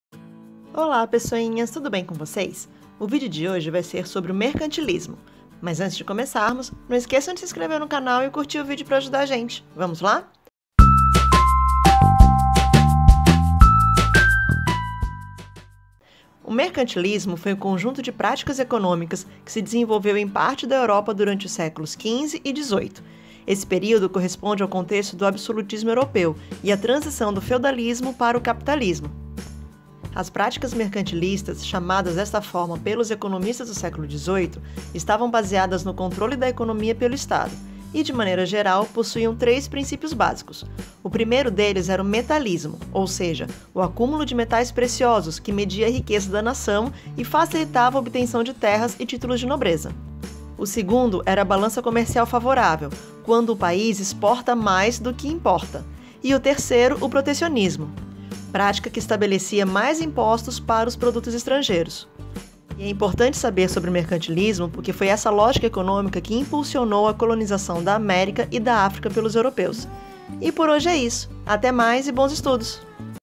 MERCANTILISMO - Ensino Fundamental
O mercantilismo está associada a lógica econômica da colonização do século XVI. Nesse vídeo você vai entender um pouco mais sobre ele!